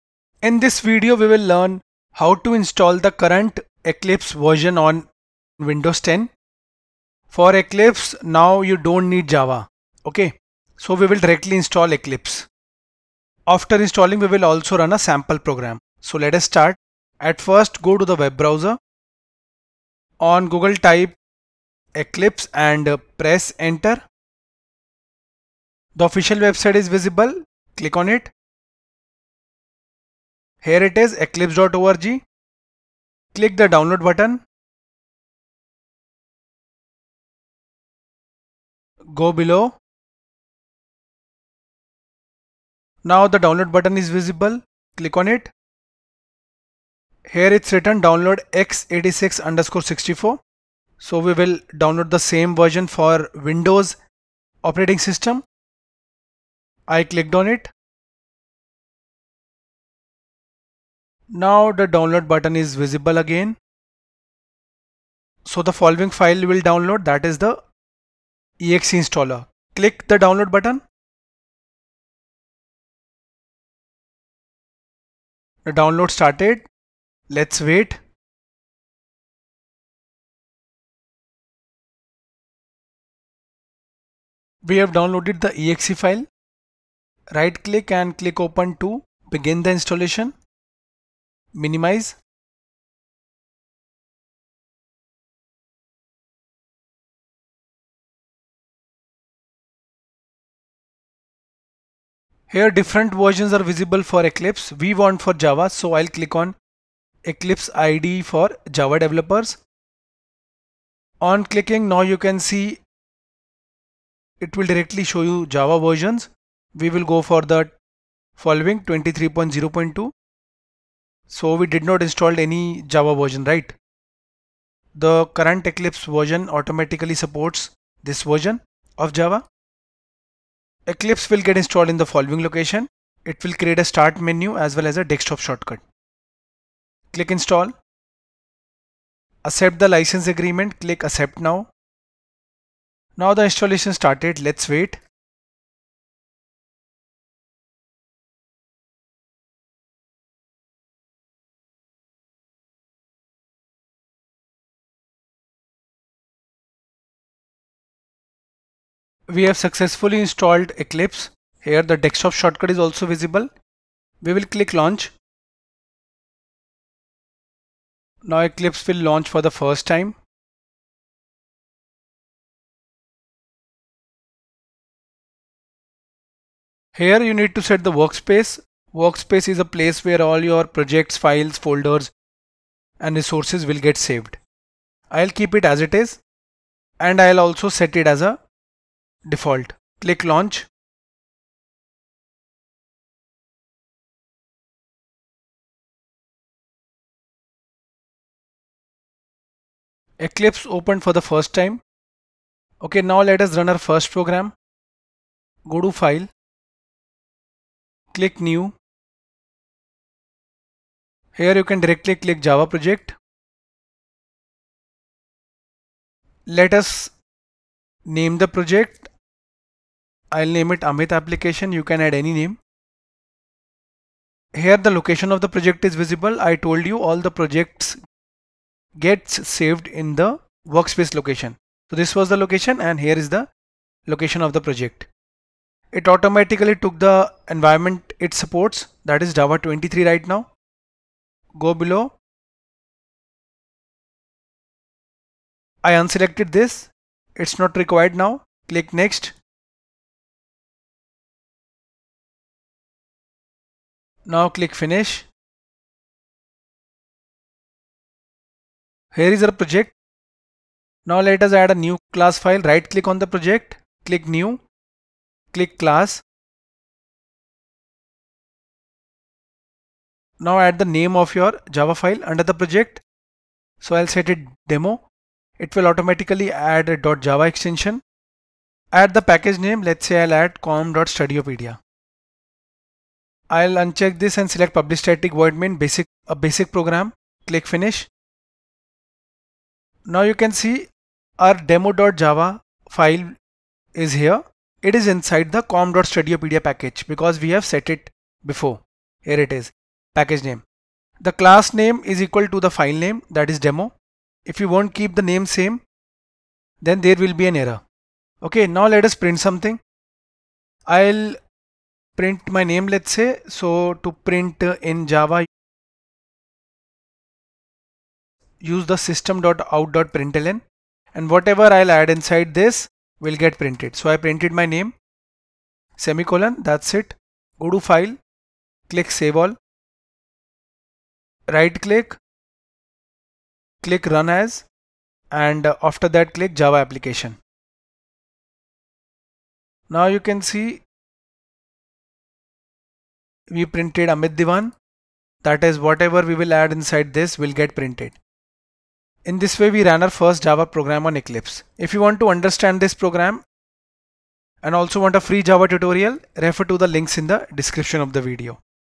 How to install Eclipse IDE 2025-06 on Windows 10
Learn to download and install Eclipse IDE with Java on Windows 10. We will also run a sample Java program after installing Eclipse.